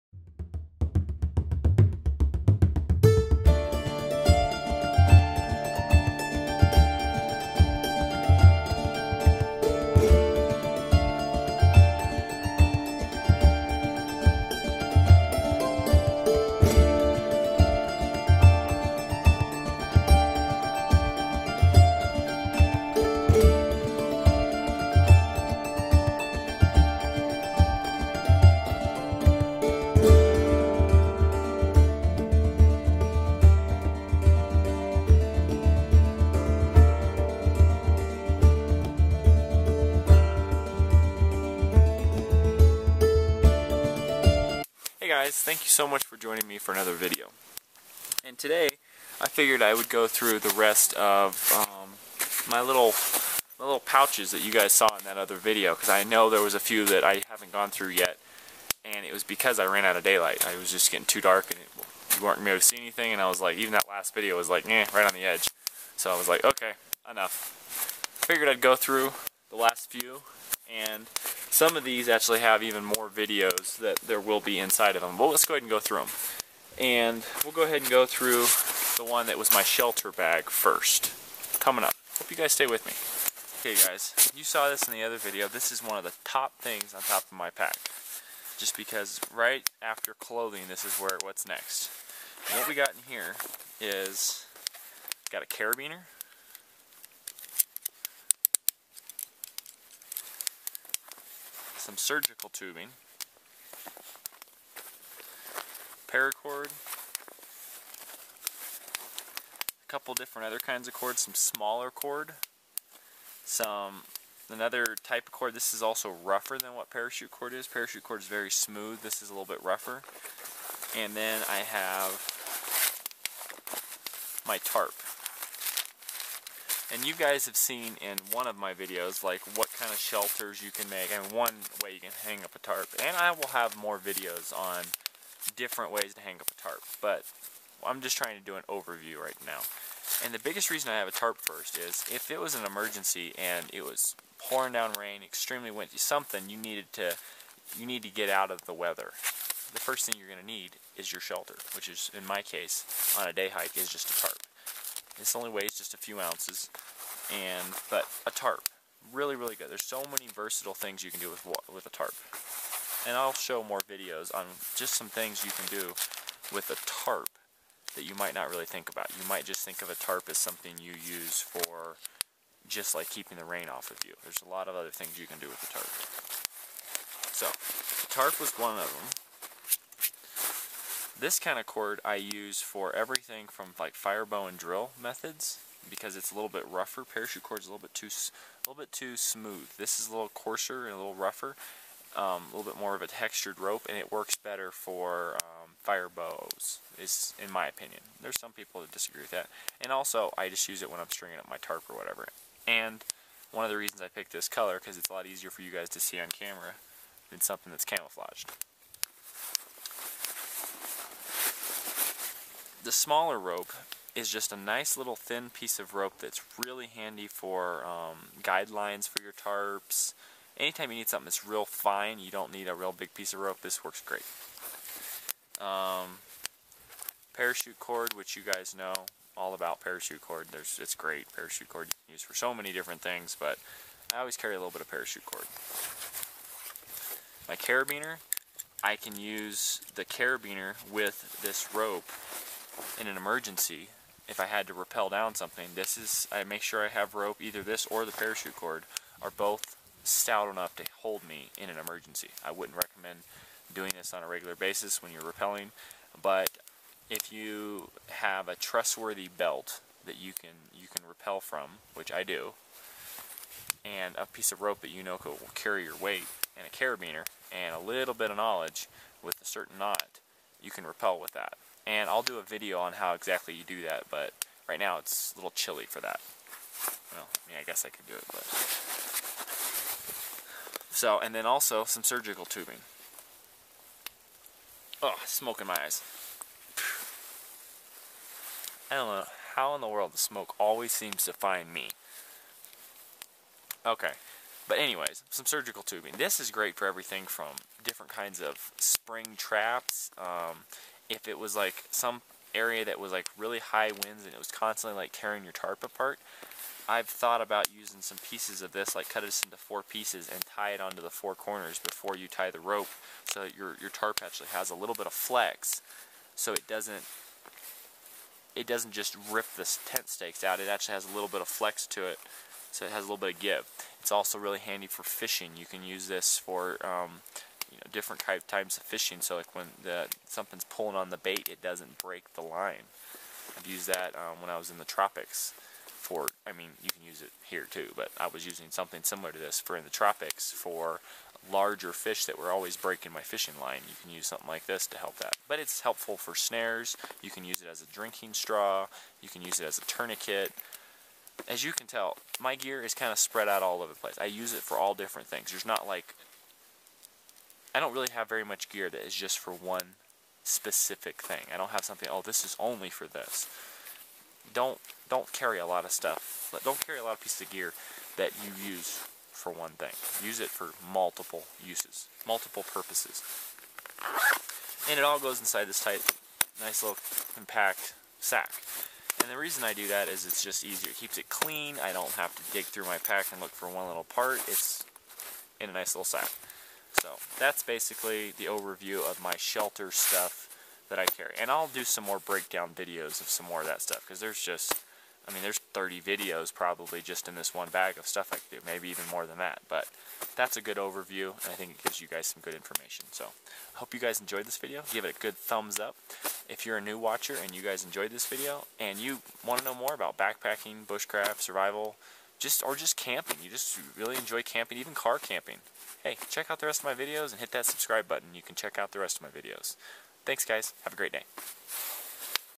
Backpack Survival Shelter Bag
Backpack Survival Shelter Bag. What type of gear I carry when I go backpacking.